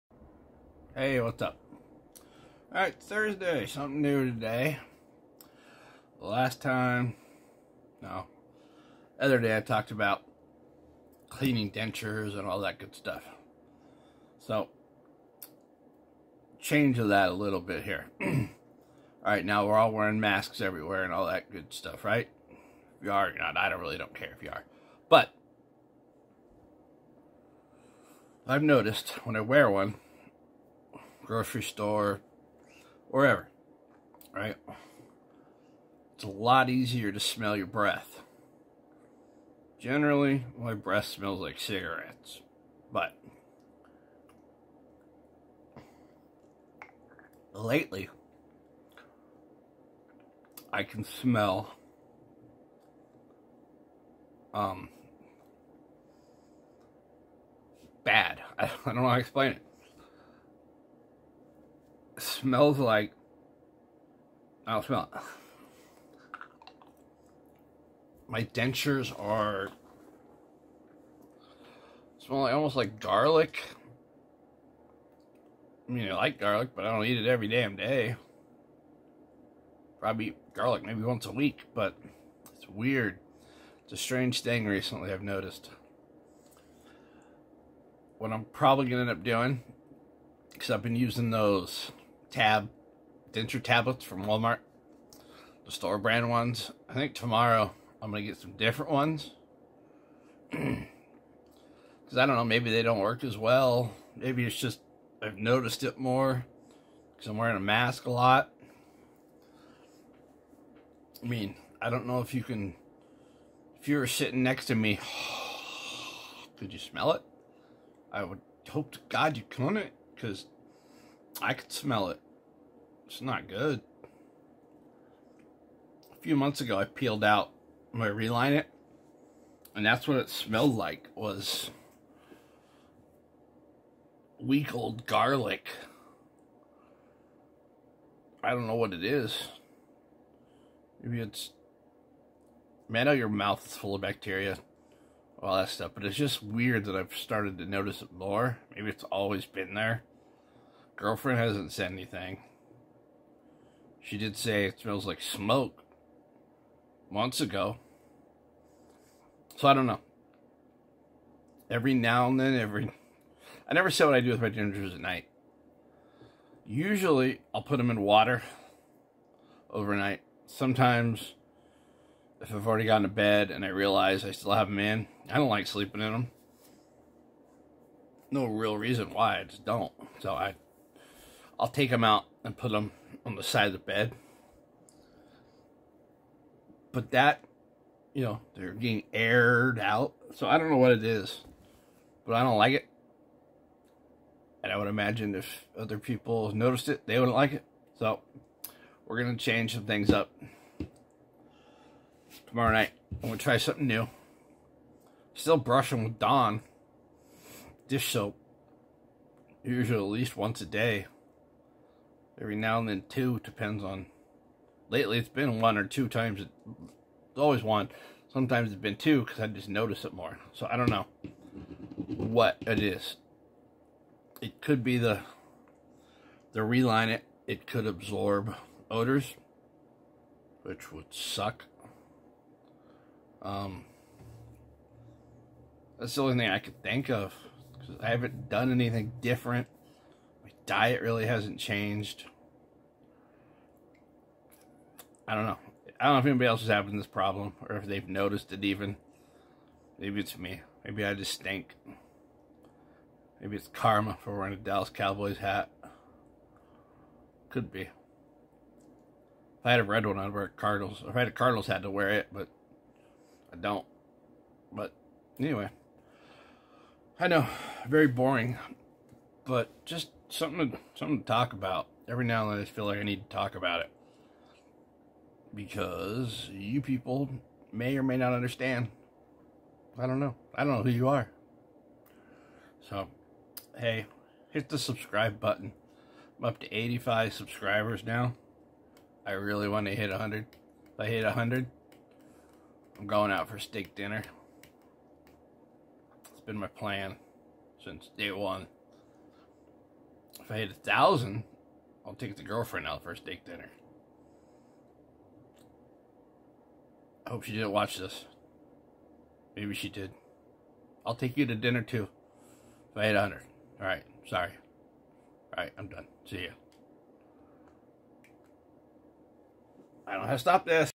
Denture stank? Time to switch it up.
I've noticed recently that my dentures smell funky. Going to try a new cleaning routine.